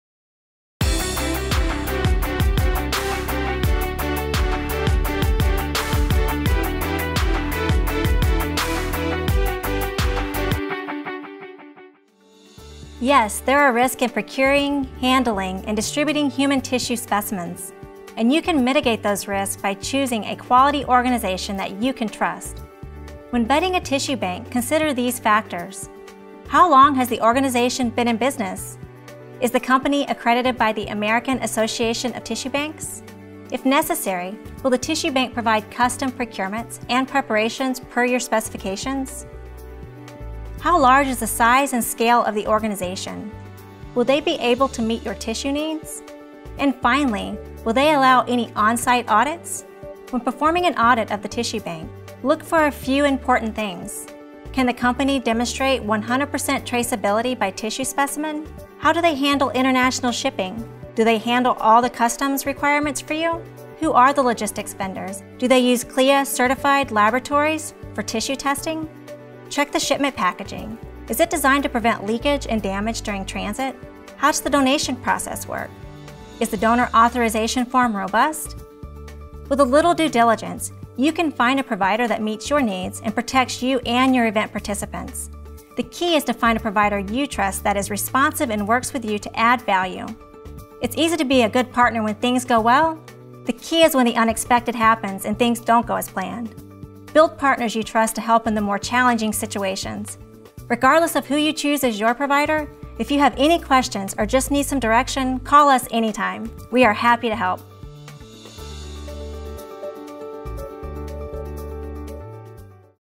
CLIENTS - How to Find a Trustworthy Tissue Provider
Learn more about vetting a tissue bank to meet your order requirements while managing associated risks.